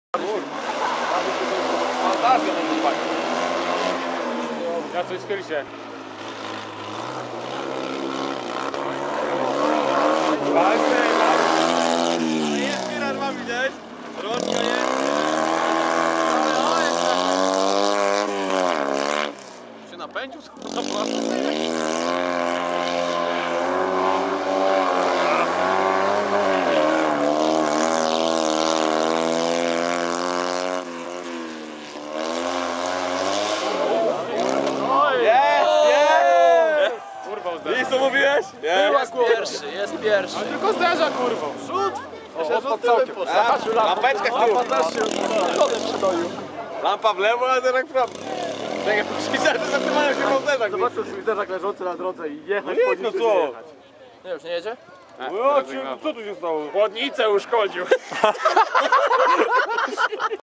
3 KJS Austeria Krokus 2011 Fiat 126p "Chłodnicę uszkodził :D"
Przejazd próby nr 3- nadleśnictwo, zakończonej lekką przygodą załogi skwintowaną przez jednego z kibiców czymś co może kandydować na rajdowego mema.